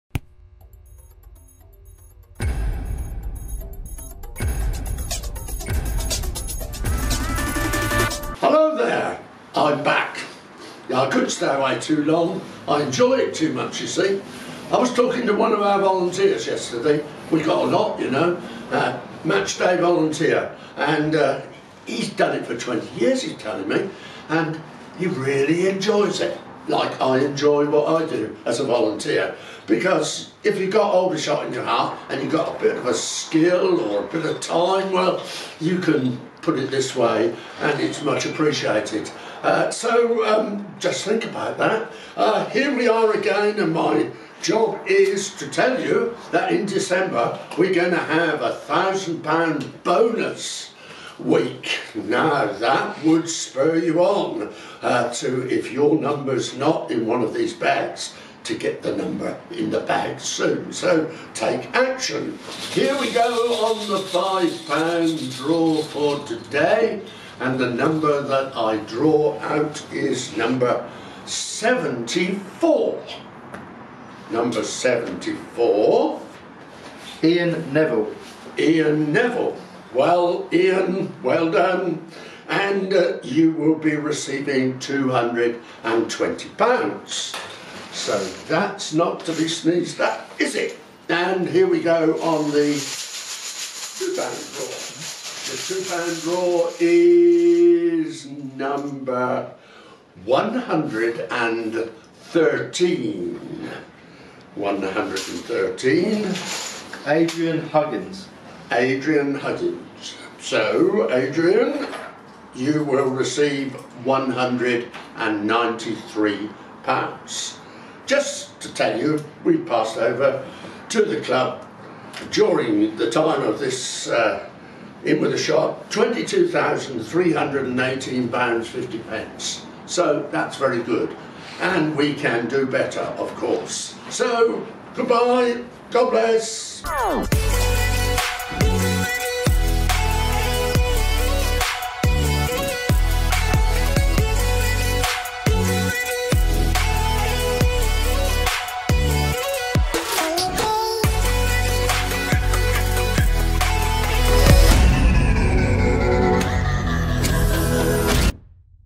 IN WITH A SHOT: Week 89 Winners! (8th October 2021)
The winners of Week 89 Winners (8th October 2021) have been announced by Club Chaplain Mike Pusey!